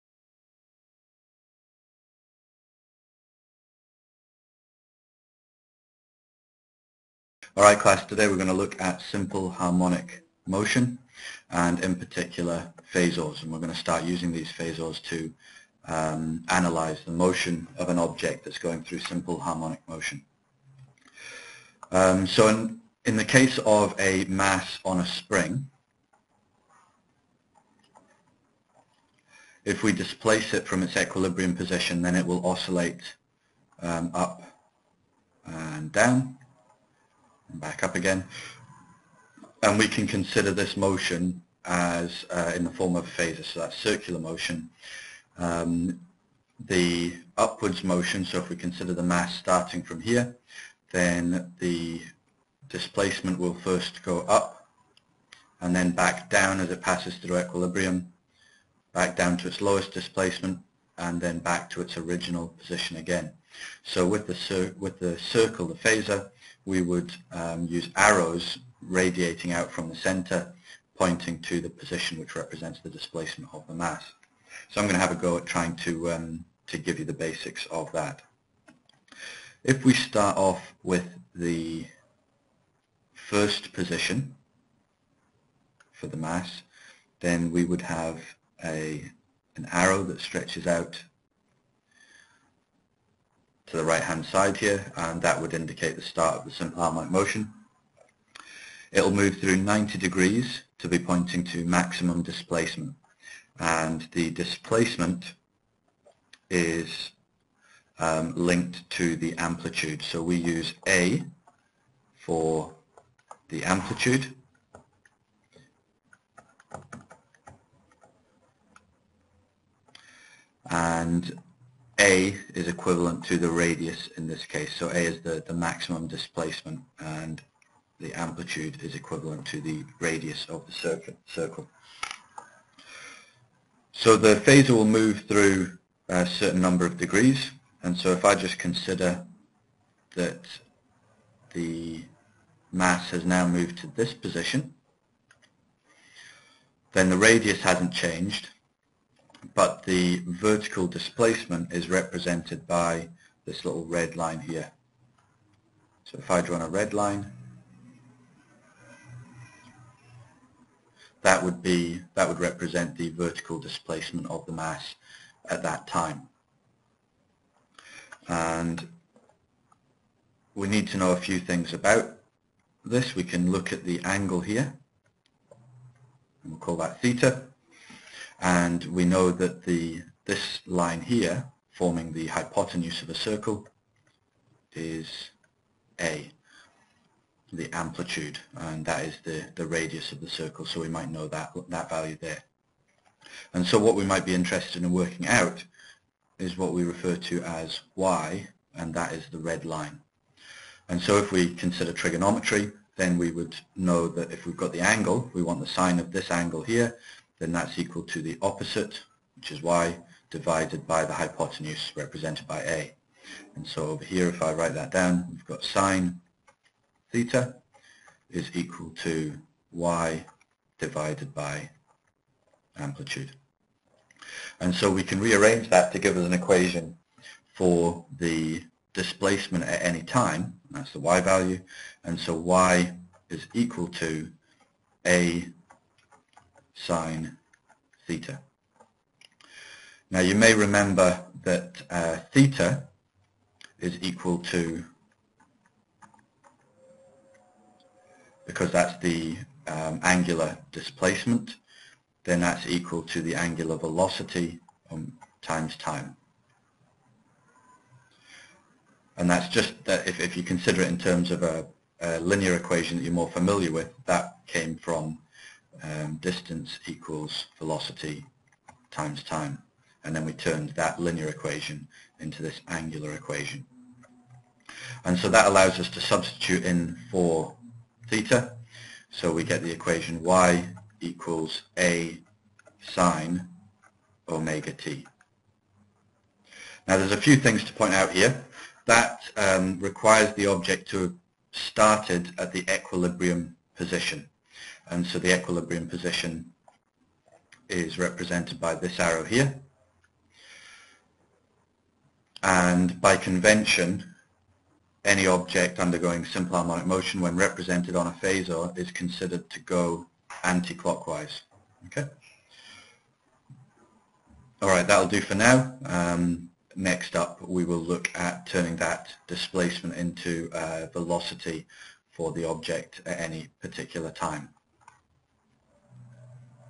Simple Harmonic Motion - finding displacement
Establishing how to find the displacement of an object undergoing simple harmonic motion using a phasor